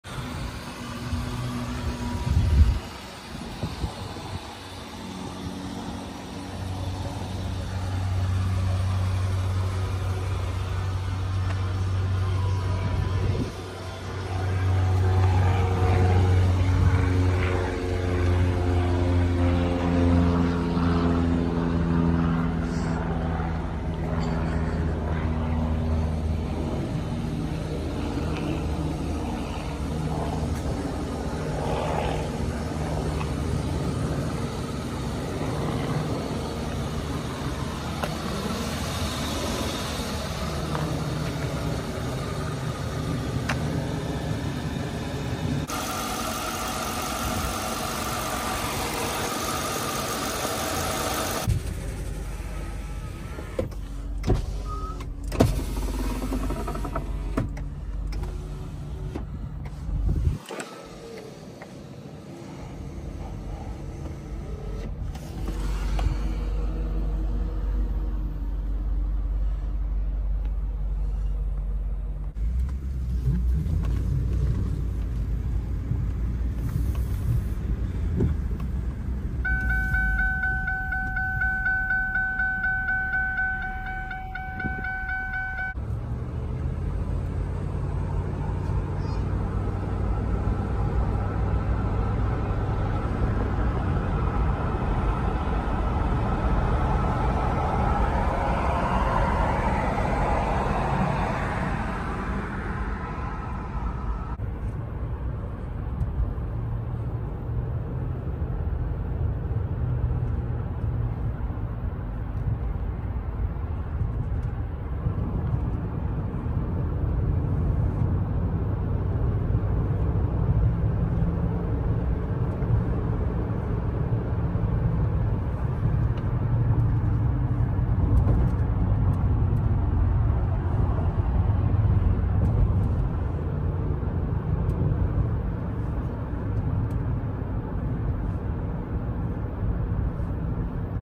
2001 LINCOLN CONTINENTAL 2 OWNER FLORIDA CAR FOR SALE
2001 LINCOLN CONTINENTAL 2 OWNER Florida car garage kept fully loaded ￼ you do not find them like this anymore ASKING $7900 ￼￼ please call to check availability
VIN: 1LNHM97V71Y667398
4.6L V8 F DOHC 32V